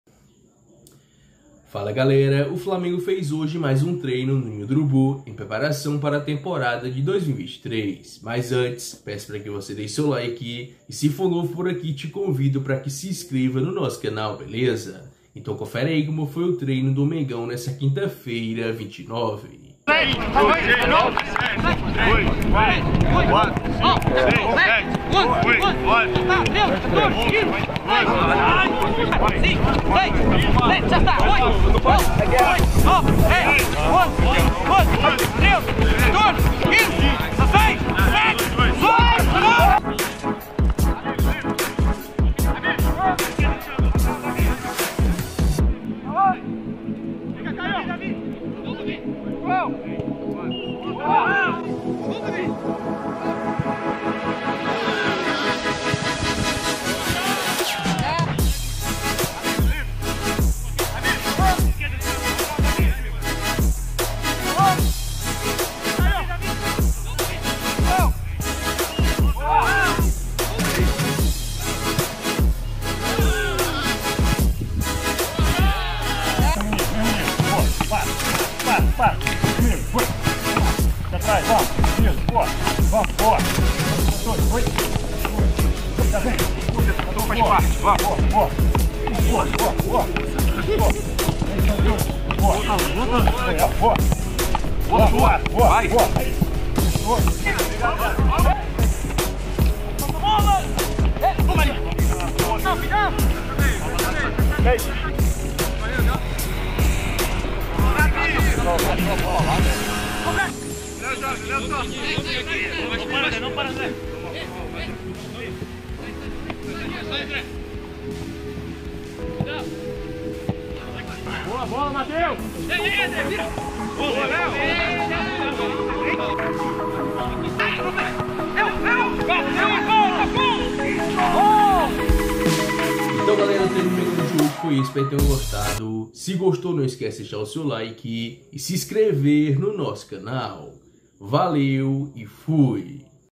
Treino do Flamengo hoje 29/12/22 | Time segue preparação com força máxima para a temporada de 2023
Fala Nação, O Flamengo segue a preparação com força máxima para a temporadade 2023. Confere ai como foi o treino do Mengão nessa quinta-feira (29).